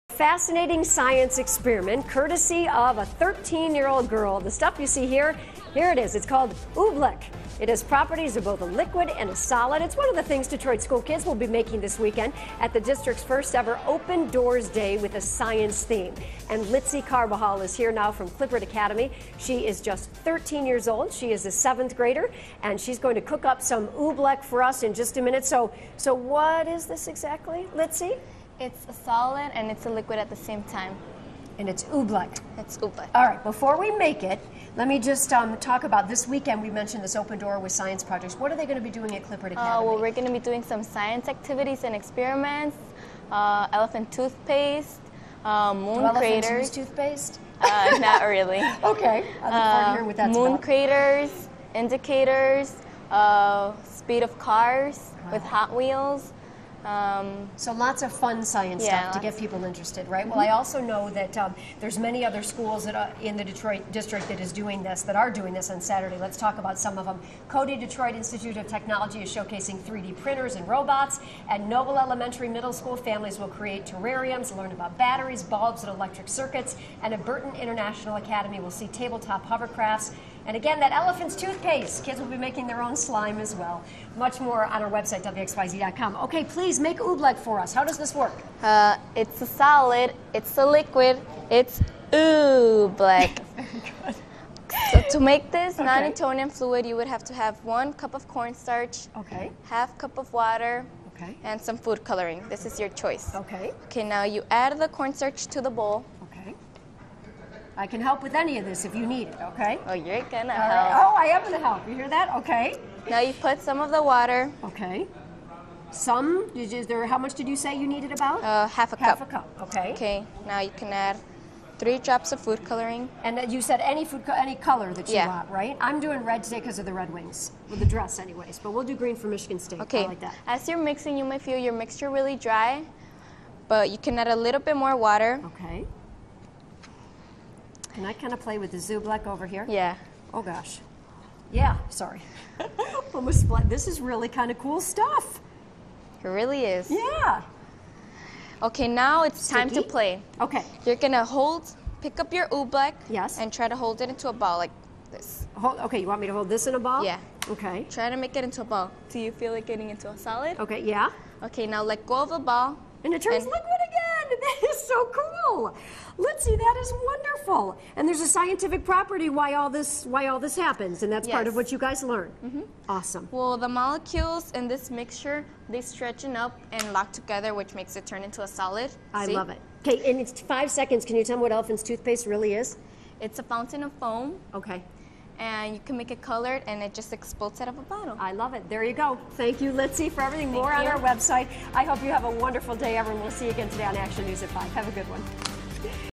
Detroit Schools celebrate science
Detroit Schools celebrate science. Teen girls makes "oobleck" with JoAnne Purtan